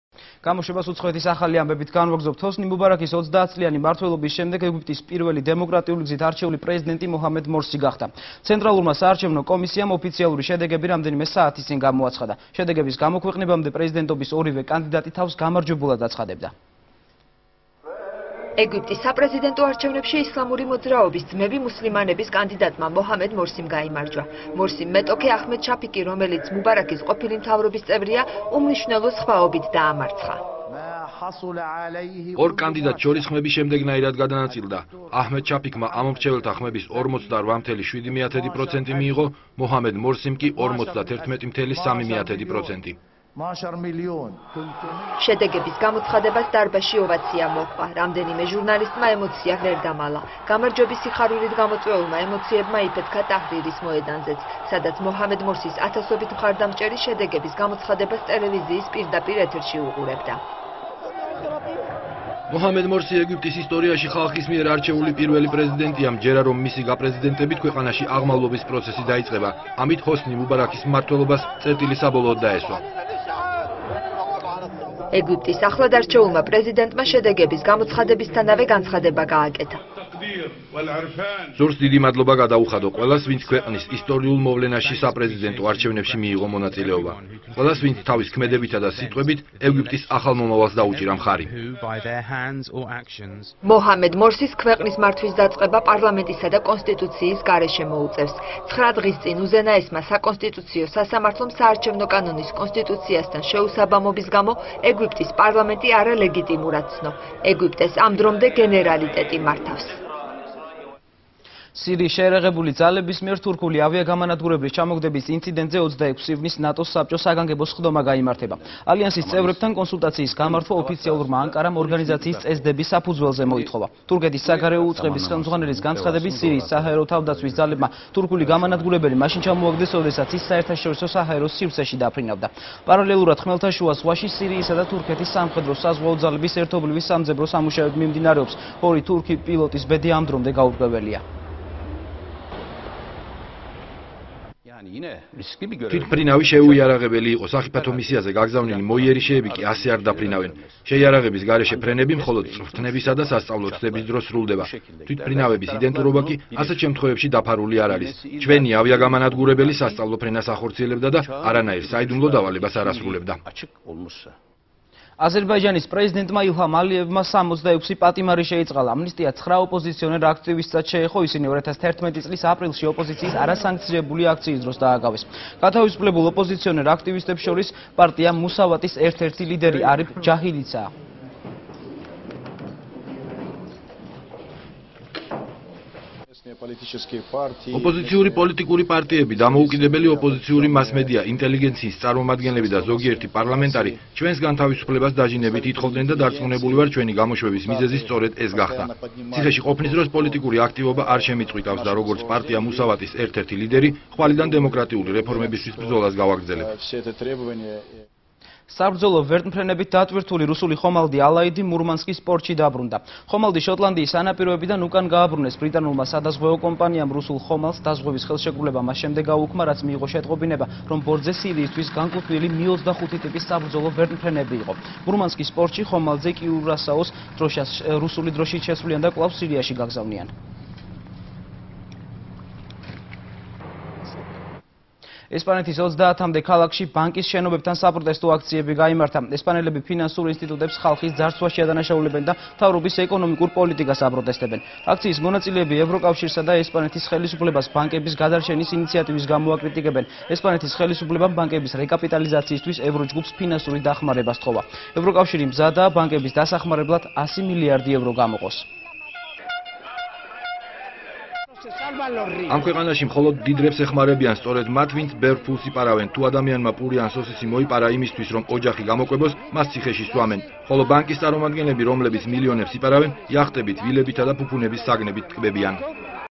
ახალი 9 | უცხოეთის ამბები | 24.06.12
ჰოსნი მუბარაქის 30 წლიანი მმართველობის შემდეგ ეგვიპტის პირველი დემოკრატიული გზით არჩეული პრეზიდენტი მოჰამედ მორსი გახდა.
სირიის შეირაღებული ძალების მიერ თურქული ავიაგამანადგურებლის ჩამოგდების ინციდენტზე 26 ივნისს ნატოს საბჭოს საგანგებო სხდომა გაიმართება.
აზერბაიჯანის პრეზიდენტმა ილჰამ ალიევმა 66 პატიმარი შეიწყალა.
საბრძოლო ვერტმფრენებით დატვირთული რუსული ხომალდი ,,ალაიდი" მურმანსკის პორტში დაბრუნდა.